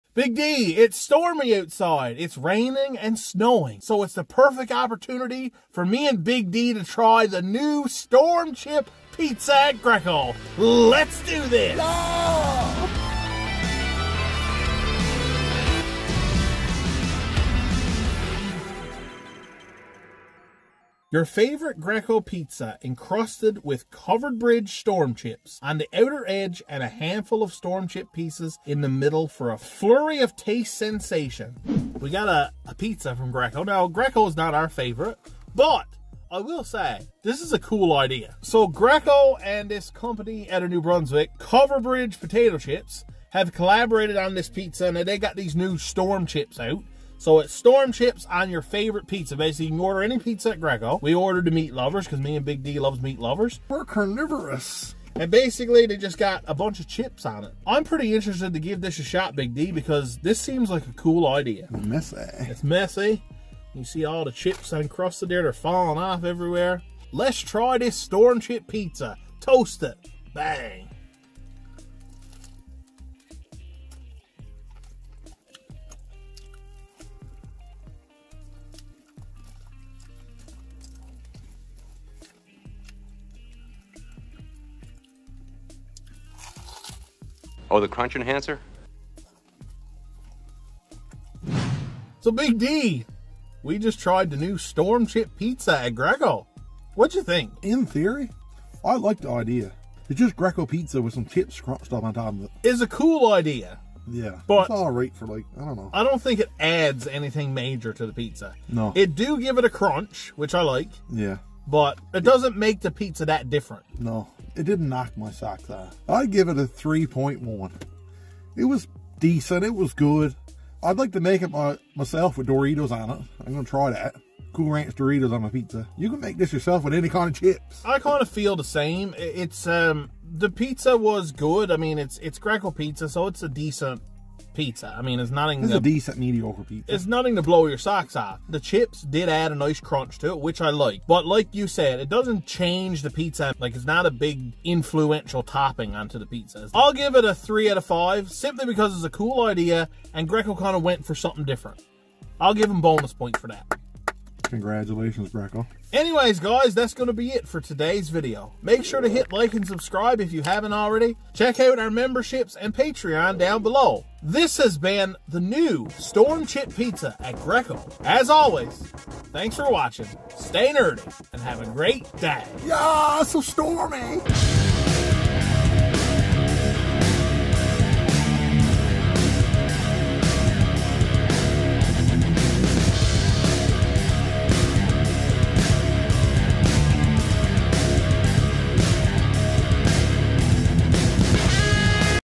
NEW Greco Storm Chips Pizza With Covered Bridge Potato Chips
Hello Foodie Friends! Today we are at Greco to try their NEW storm chip pizza! Is it any good? Let's find out!!

Check us out on Tik Tok!